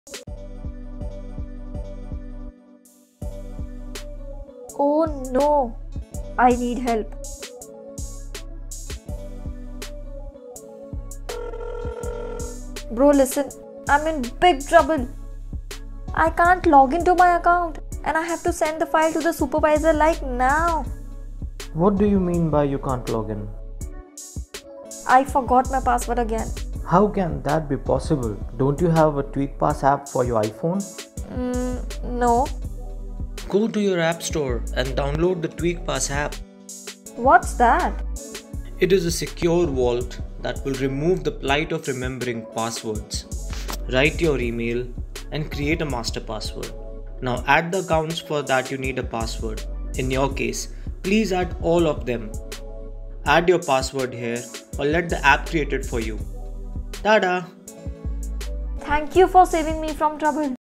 Best Password Manager: Tweak Pass for iPhone
Do you have trouble remembering your password? Well, how about saving your passwords via password manager. Yes, password managers are the future of saving your password and living a stress-free life. Afterall, forgetting your password can be so troublesome and risky. So here we are, to suggest to you a smart suggestion that will be your one stop destination for all your passwords. Say bye-bye to those sticky notes and hours of flipping through pages to find your password. TweakPass by Systweak is the best password vault that will help you here.